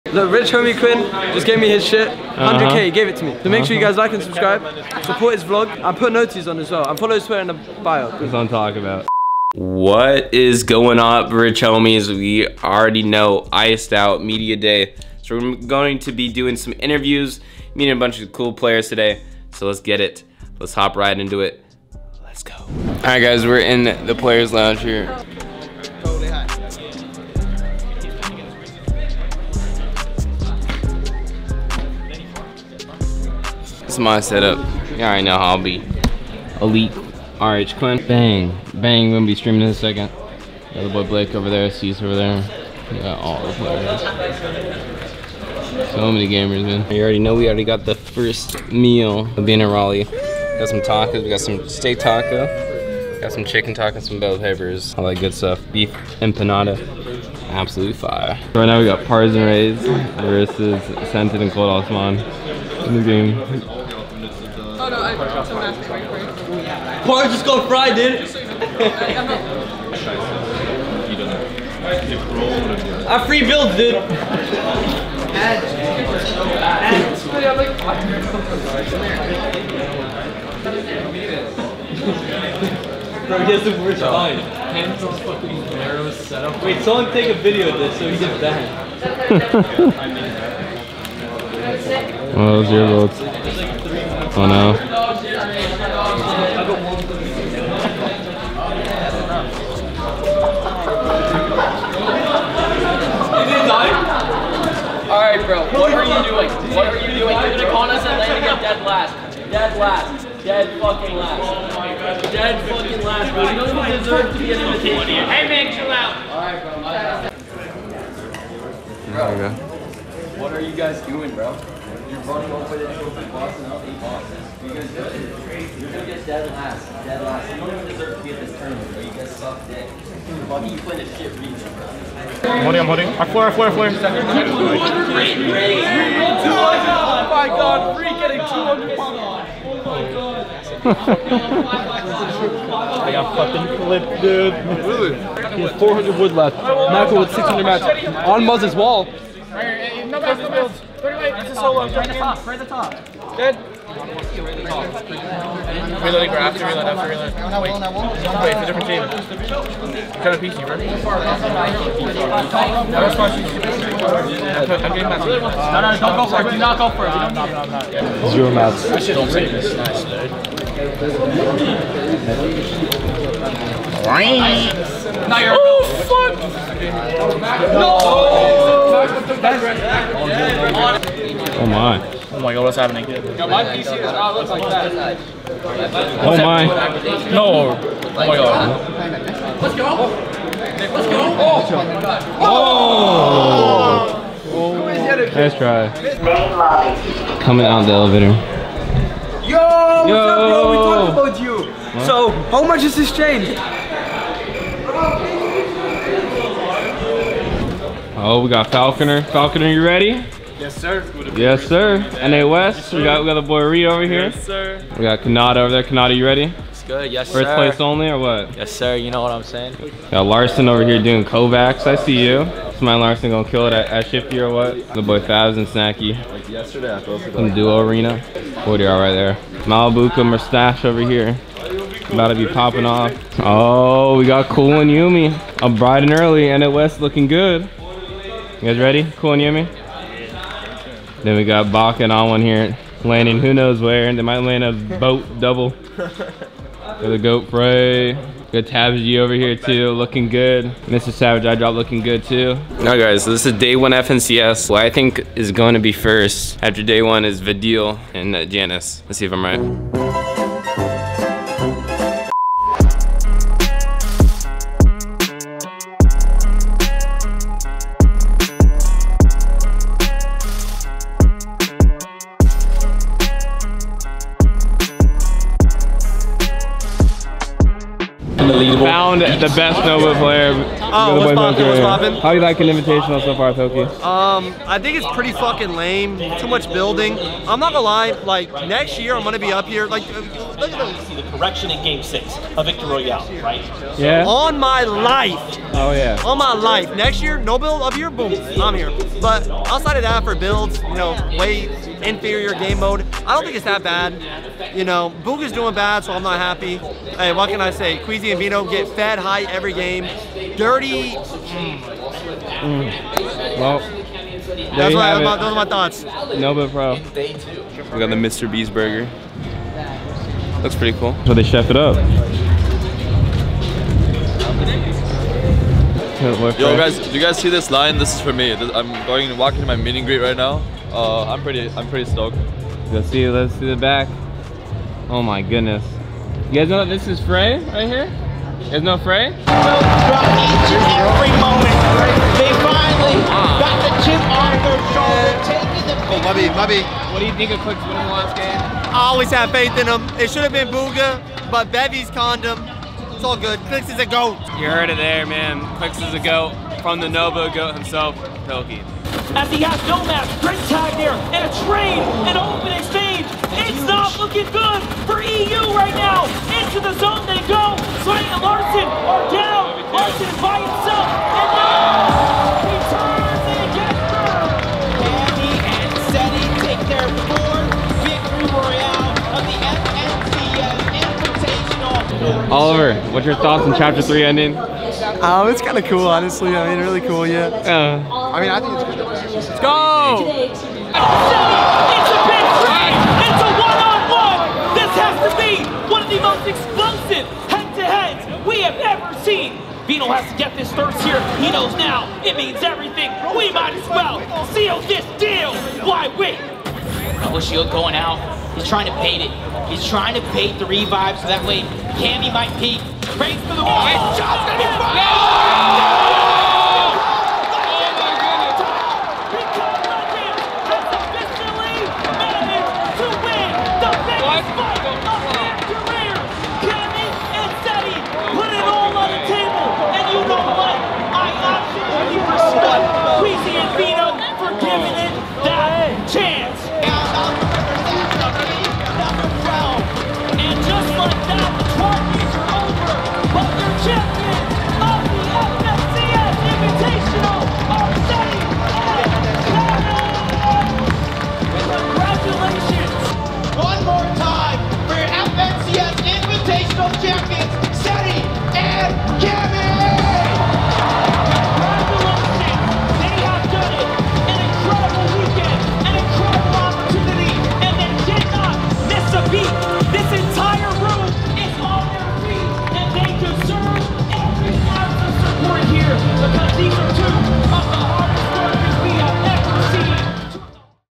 FNCS Invitational VLOG Day 1 & 2 (ft. Clix, Bugha & more)
RichHomieQuinn

Make sure to use code "Quinn" in the Fortnite item shop (ad)